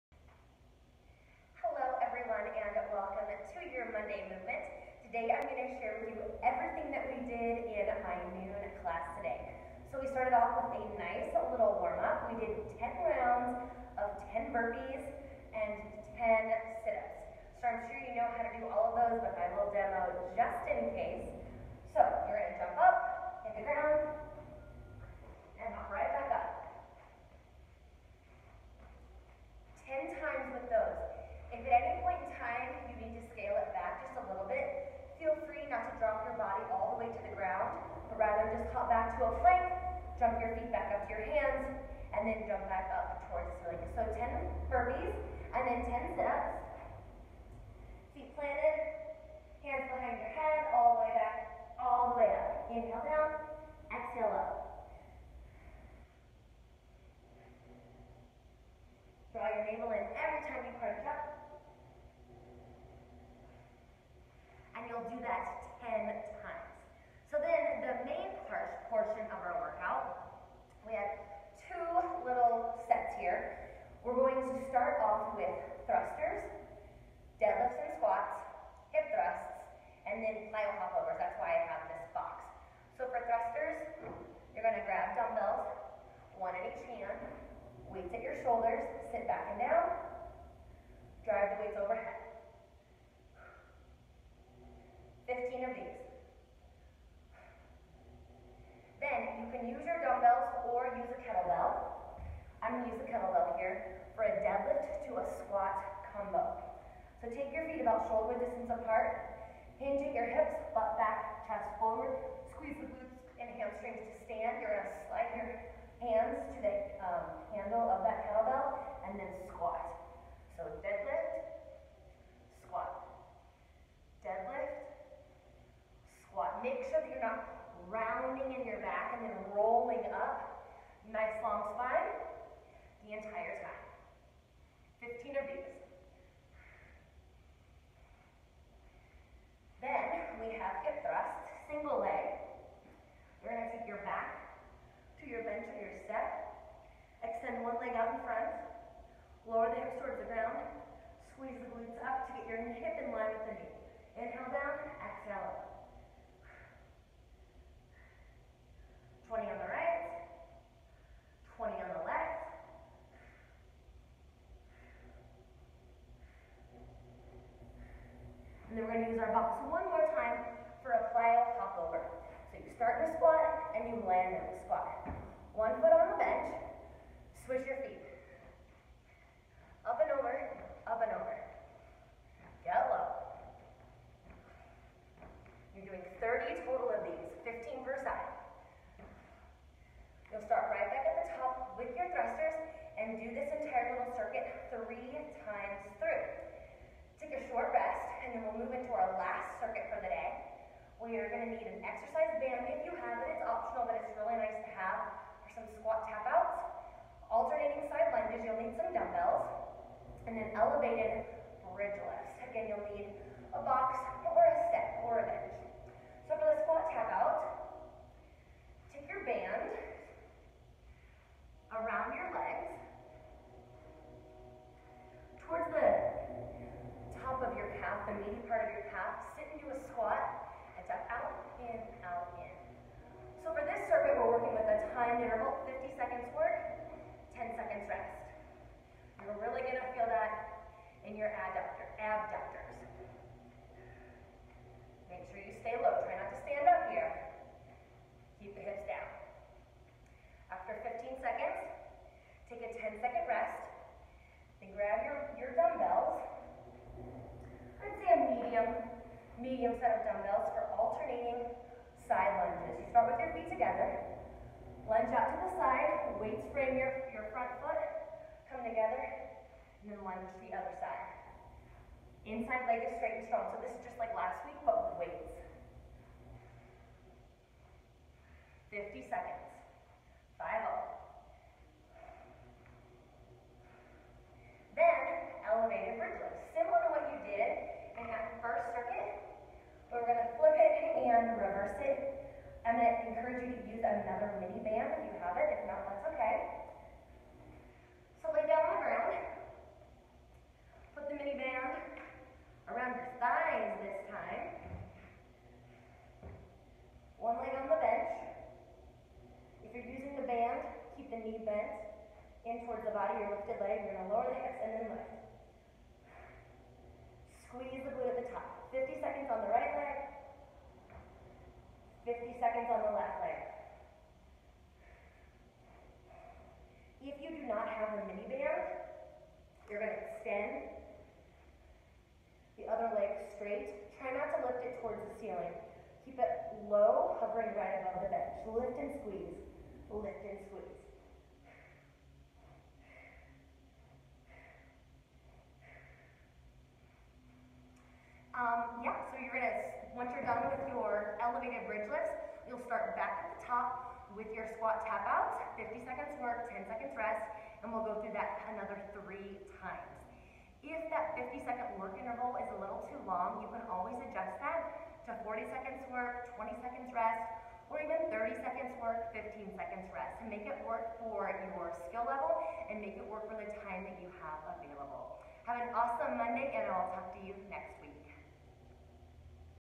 Workout Wednesday - Hump Day Circuits
Warm-up:
10 Rounds
10 Burpees
10 Sit-ups

Circuit 1:
Thrusters - 15
Deadlift to Squat - 15
Elevated Single Leg Hip Thrusts - 20/leg
Plyo Hop-over - 30 total
Repeat 3x's

Circuit 2:
:50 work/:10 rest
Squat Tap Out
Alternating Side Lunges
Elevated Single Leg Bride Lifts - Right
Elevated Single Leg Bride Lifts - Left
Repeat 3x's